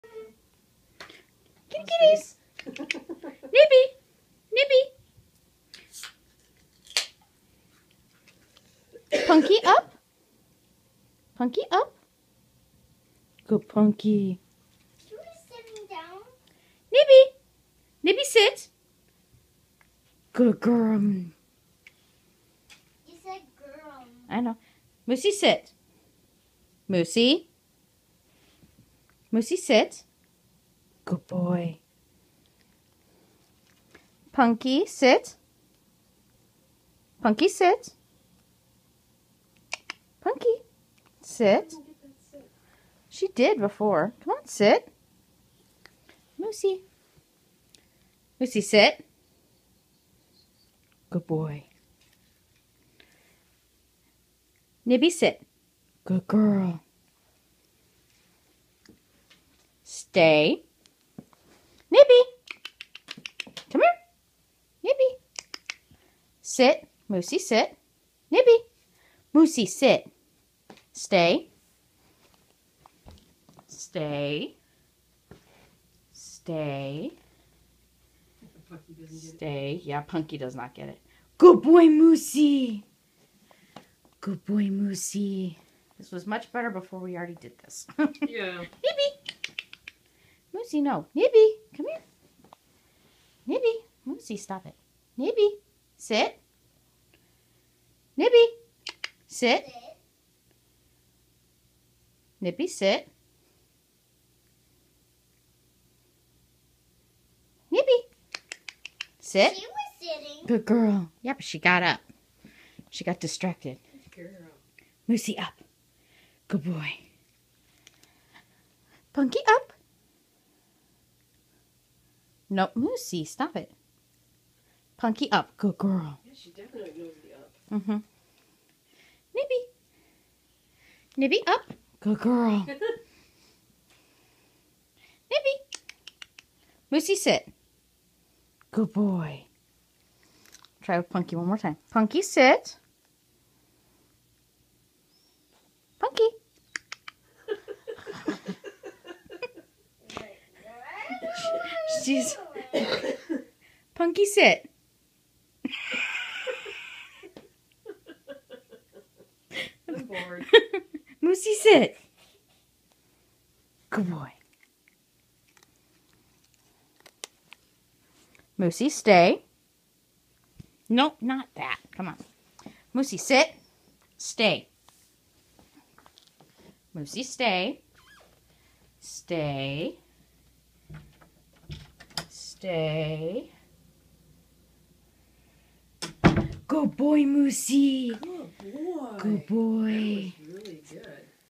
Cat training
Bare in mind, I already trained them once before I remembered to record. So by this time Nibs (black) was getting bored and distracted and Punky (orange and white) was too uppity to participate. Moose (grey and white striper) was still very cooperative.